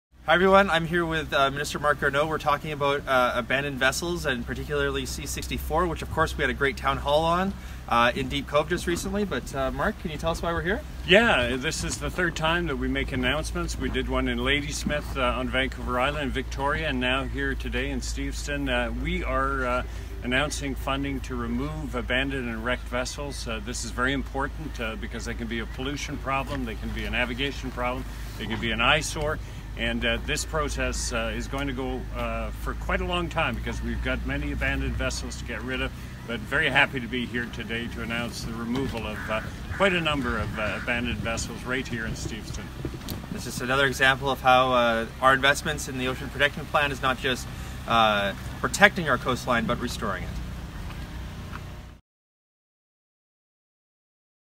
Abandoned Boats Program - Bill C-64 Passed!
Deep Cove community members... I have some exciting news to share! After our Town Hall meeting in January, real change has been happening! ✅Bill C-64 passed... giving the Port Authority more tools. ✅ The District and Port have started conversations because of you. By sharing your voice, you’re helping deliver results for North Vancouver and Burnaby. A big thank you to Councillor Lisa Muri for working with us! Let's continue to create real change for our community... by working together.